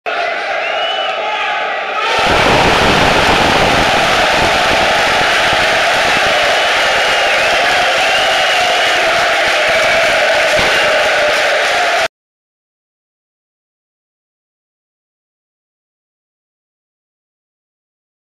Ηρακλής-ΑΕΛ 1-0 ΓΚΟΛ Κιρόφσκι 2013 by aelole.gr
Το σας παρουσιάζει. Ο Ηρακλής άνοιξε το σκορ με εύστοχη εκτέλεση πέναλτι του Κιρόφσκι. Ο Σέρεμετ προσπάθησε βρήκε τη μπάλα, αλλά δεν μπόρεσε να αποκρούσει το πέναλτι